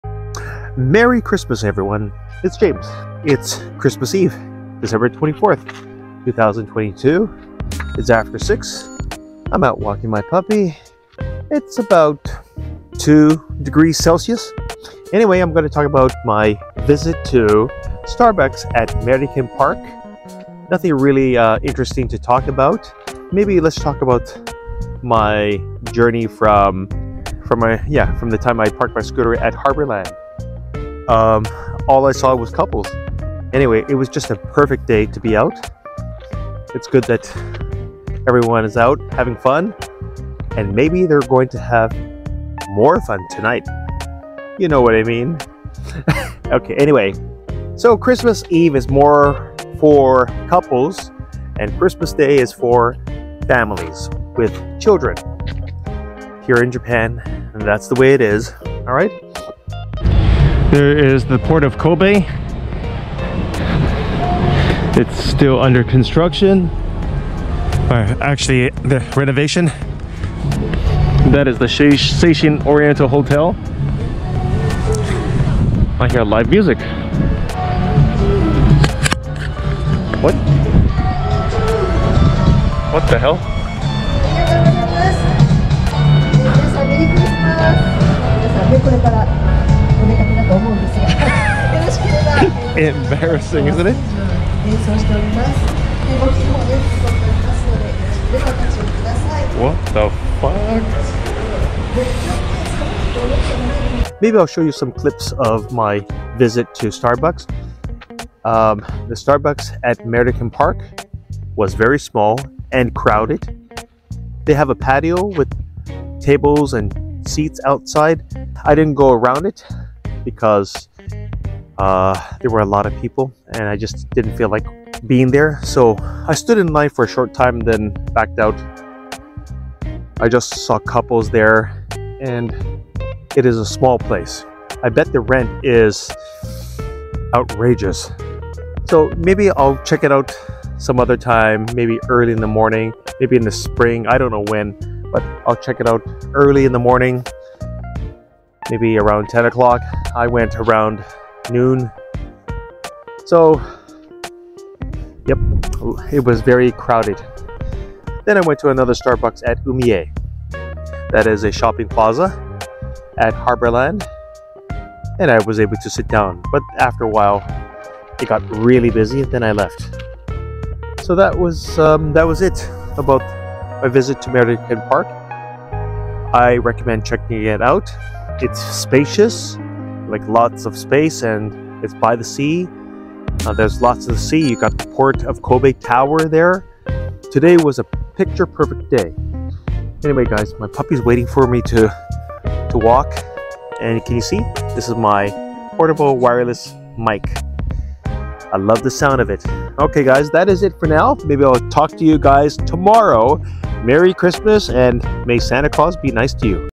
Starbucks at Meriken Park was like this on CHRISTMAS EVE
This video is about my visit to a cool Starbucks at Meriken ParkHarborland Kobe on Christmas Eve.